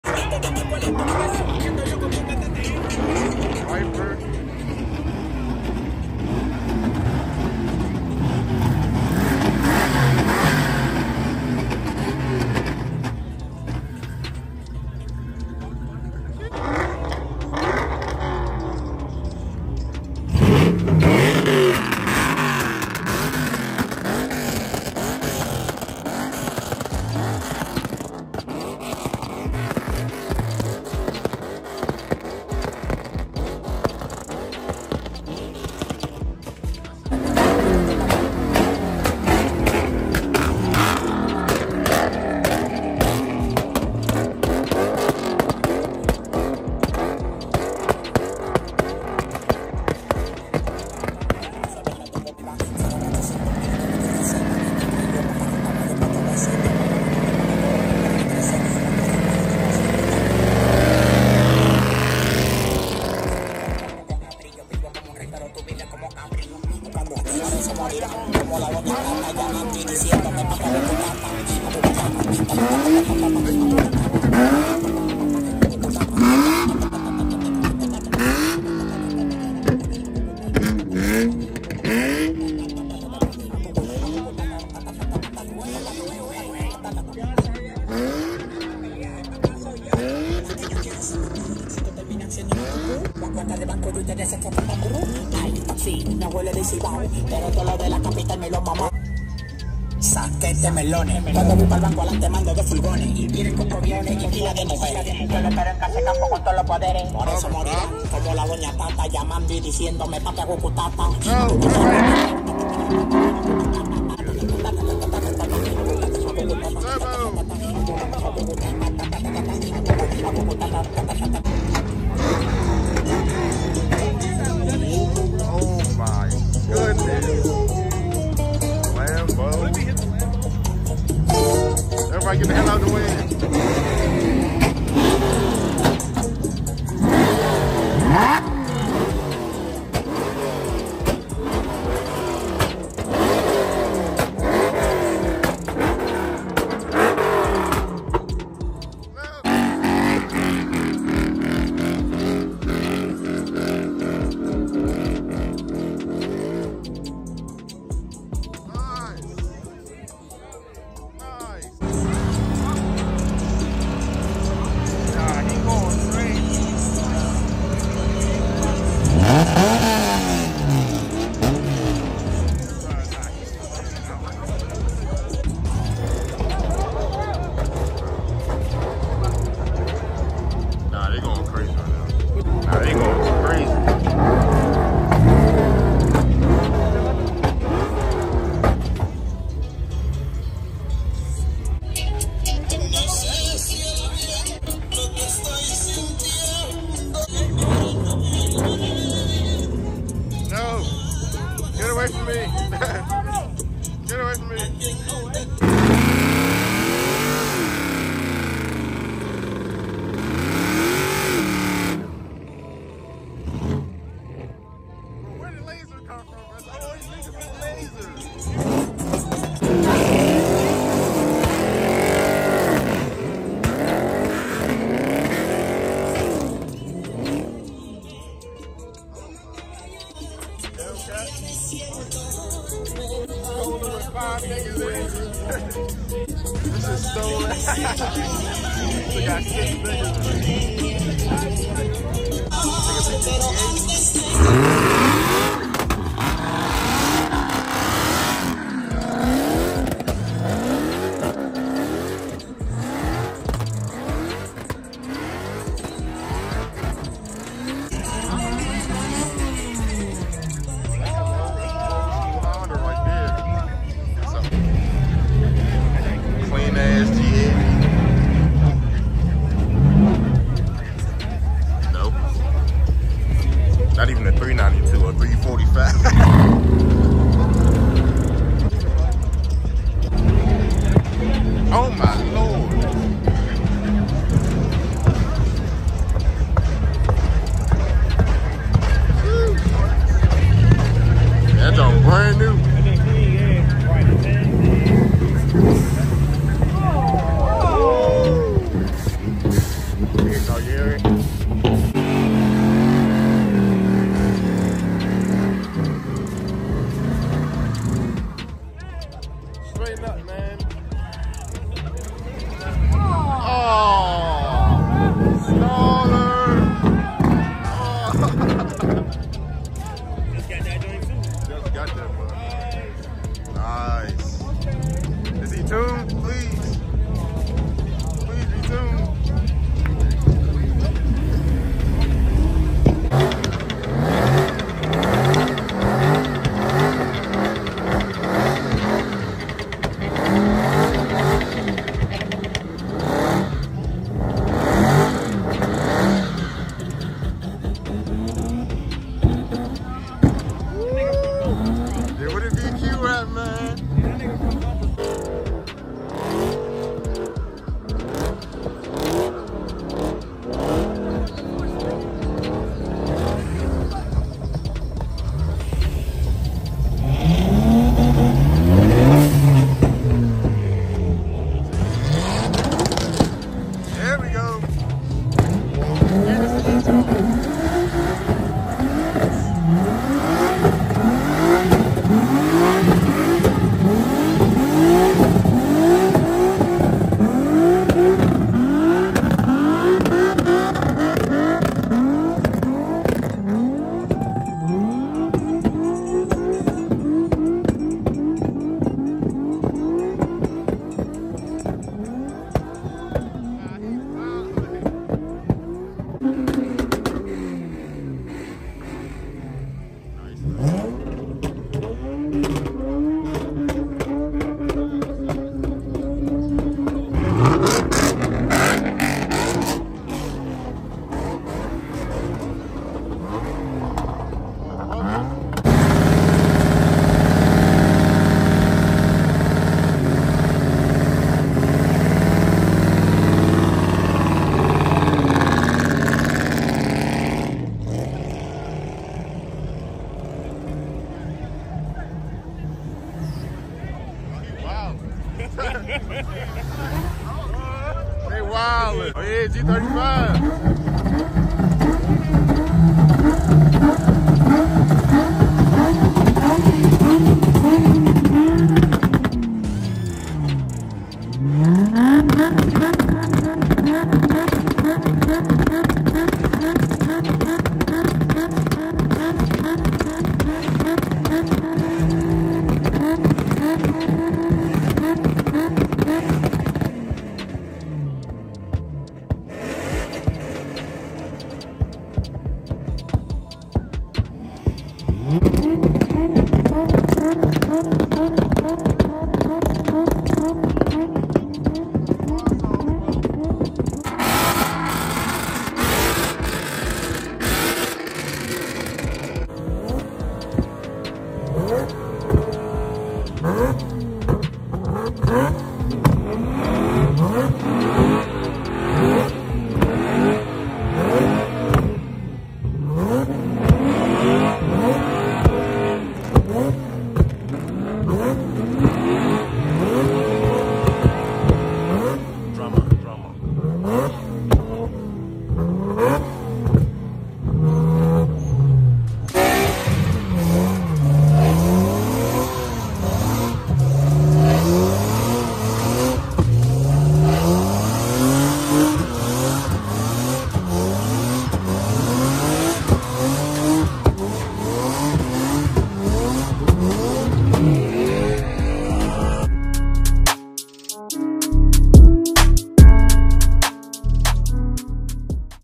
Huracan Performante Pulls Up To A BANGER!! (CRAZY BURNOUTS)
Beats: Raideronthebeat, Drxma, White Mamba, lvcien